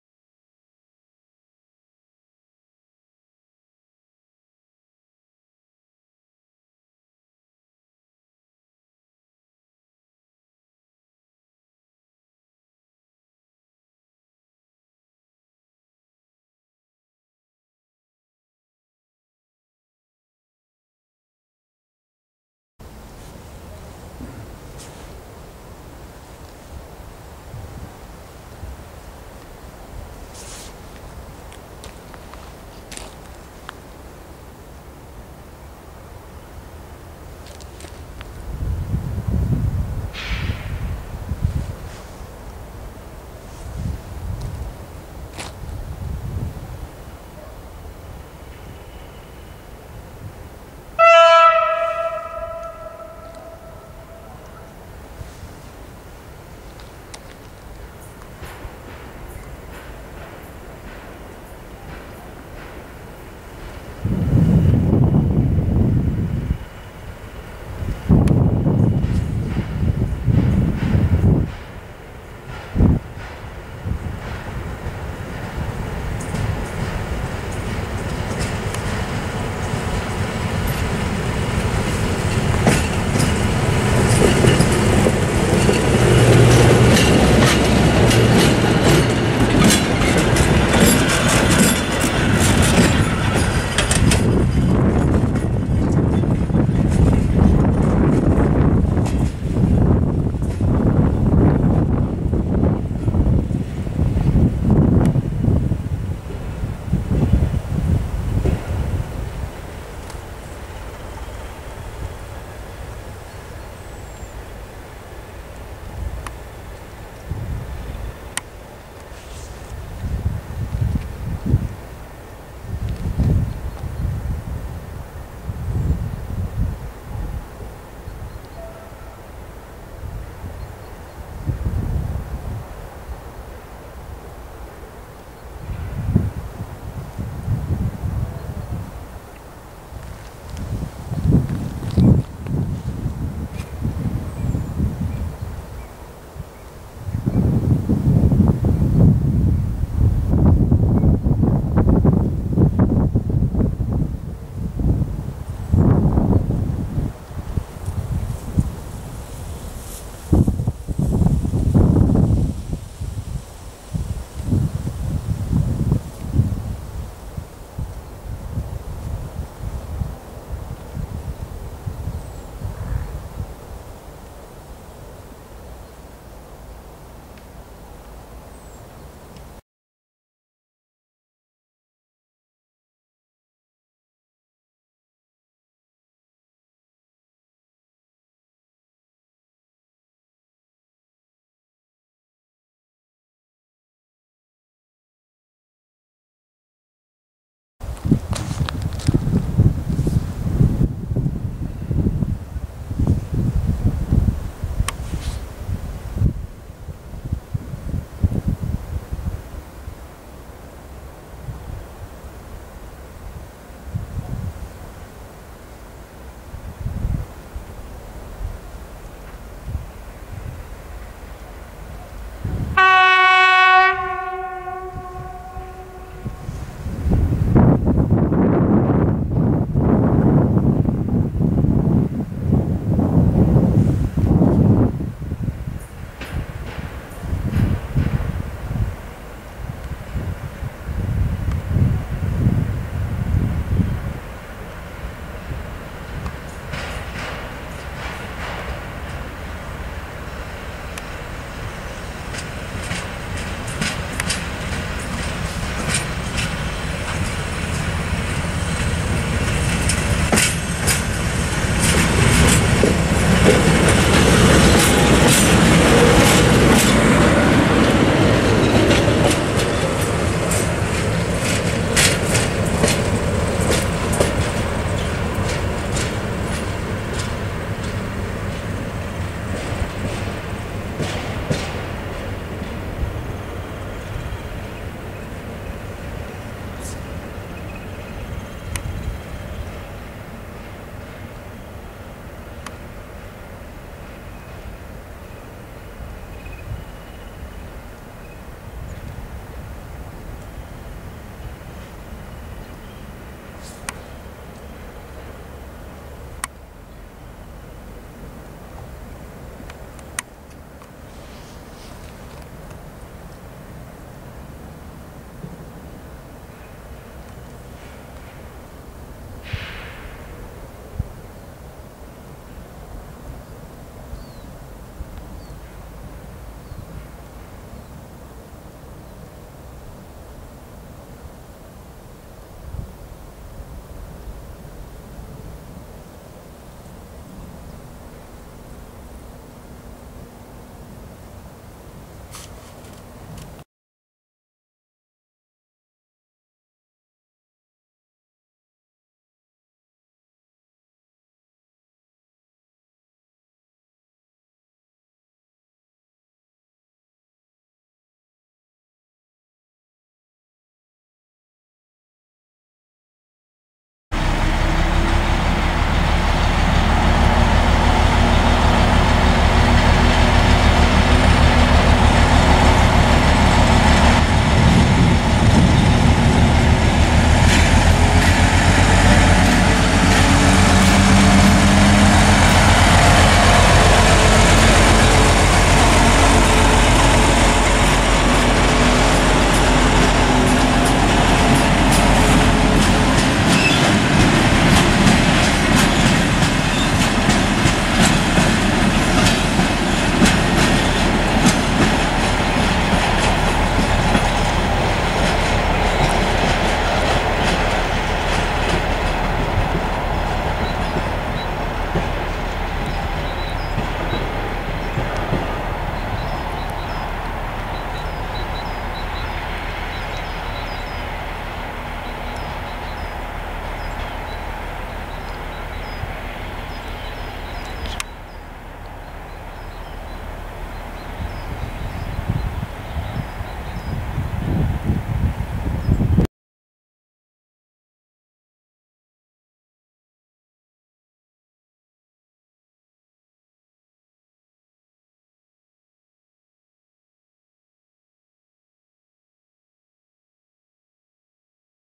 Zadymianie okolicy (En) The opacity of the area
Kompilacja dwóch krótkich składów towarowych oraz SM42 jadącej luzem:
1. SM42-338 + 2 x Es + Rp1
2. SM42-599 jedzie luzem + Rp1
3. T448P-093 + 4 x Zas
Nagranie wykonane 23 marca 2011 w okolicach stacji rozrządowej Poznań Franowo
(En)
Compilation of two short freight trains and a diesel locomotive was coming loose:
1. SM42-338 + 2 x cars type Es + horn
2. SM42-599 going bulk + horn
3. T448p-093 + 4 x cars type Zas
The recording was performed March 23, 2011 in the vicinity of Poznan Franowo Yard